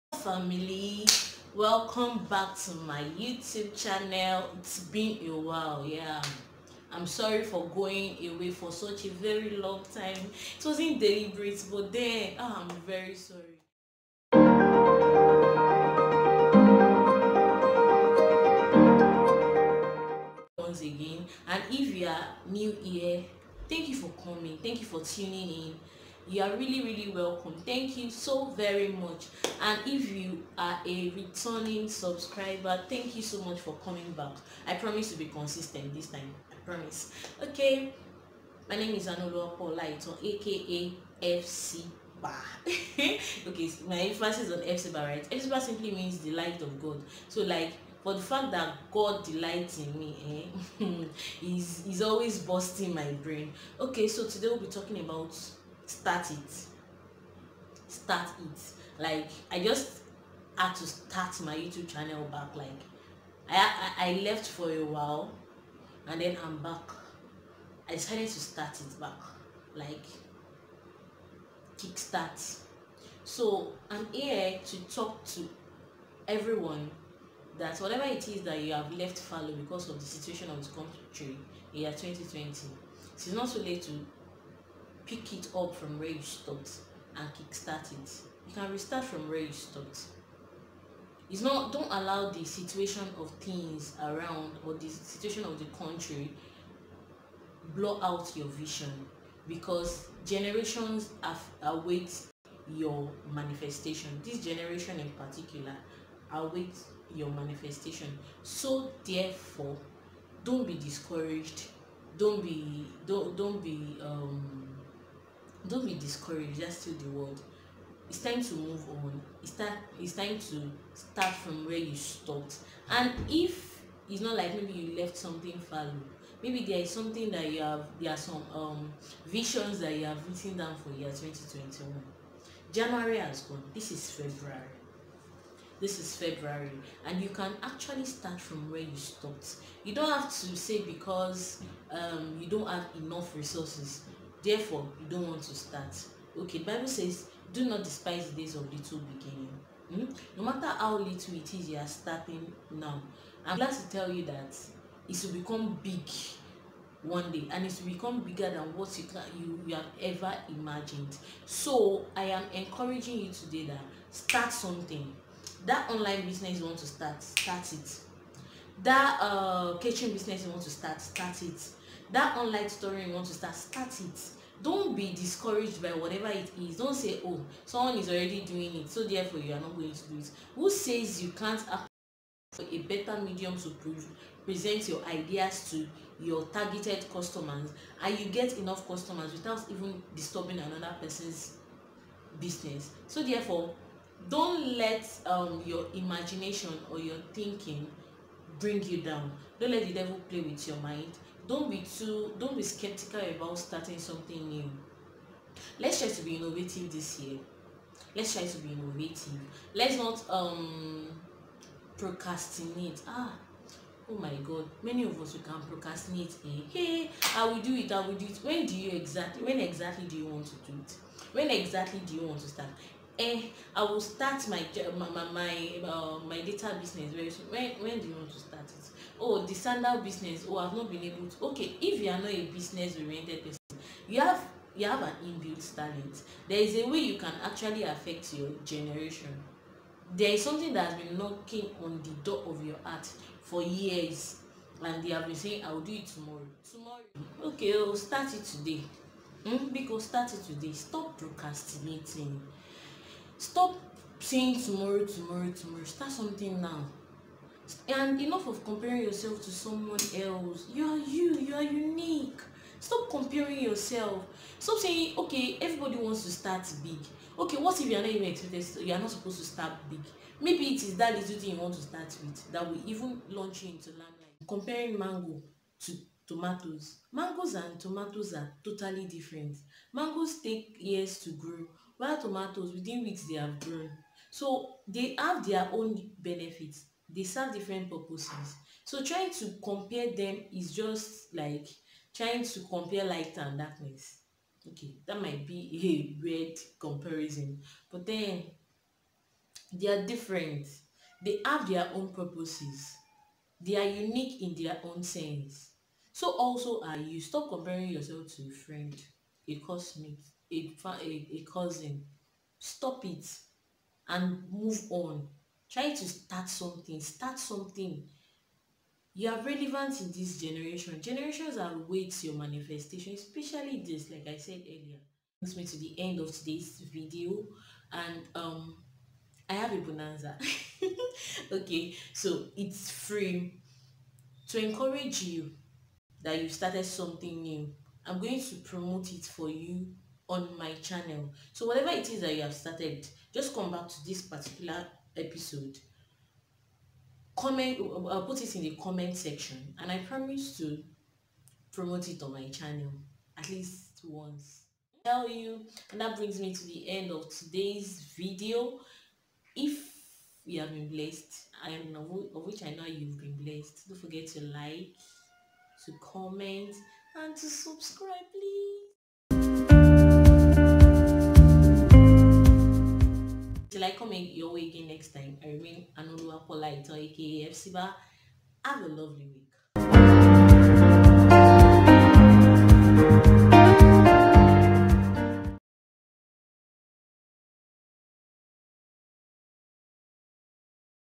START IT!
Hello Guys!

Its really great having you back in this space.

My sincere apologies for going away for such a long time!

I promise to keep this space moving with back to back content on faith, lifestyle, purpose/goals and relationship with the help of God and you (your views, likes, comments, shares and subscription keep me going)

I'm a writer, I decided to bring to life my thoughts.

Don't forget to start something today, enough of procrastination, this generation awaits your manifestation.

Guess what? I have a bonus for you at the end of this video. Make sure you see to the end.